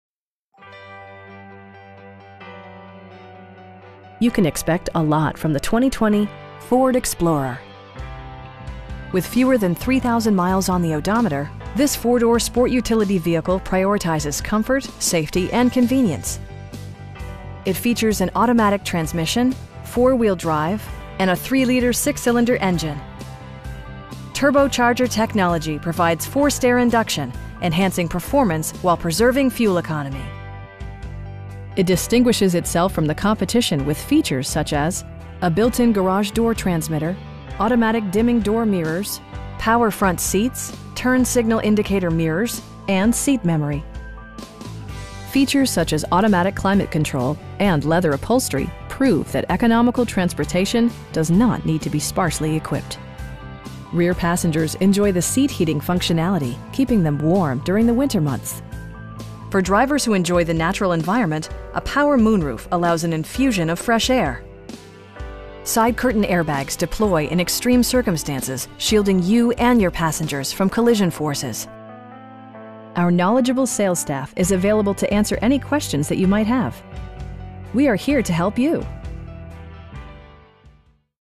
2020 Ford Explorer Platinum in Fenton, MI 48430
Lasco Ford
2525 Owen Road

You can expect a lot from the 2020 Ford Explorer. With fewer than 3,000 miles on the odometer, this 4 door sport utility vehicle prioritizes comfort, safety and convenience. It features an automatic transmission, 4-wheel drive, and a 3 liter 6 cylinder engine. Turbocharger technology provides forced air induction, enhancing performance while preserving fuel economy. It distinguishes itself from the competition with features such as: a built-in garage door transmitter, automatic dimming door mirrors, power front seats, turn signal indicator mirrors, cruise control, an overhead console, and seat memory. Features such as automatic climate control and leather upholstery prove that economical transportation does not need to be sparsely equipped. Rear passengers enjoy the seat heating functionality, keeping them warm during the winter months. For drivers who enjoy the natural environment, a power moon roof allows an infusion of fresh air. Side curtain airbags deploy in extreme circumstances, shielding you and your passengers from collision forces. Our knowledgeable sales staff is available to answer any questions that you might have. They'll work with you to find the right vehicle at a price you can afford. We are here to help you.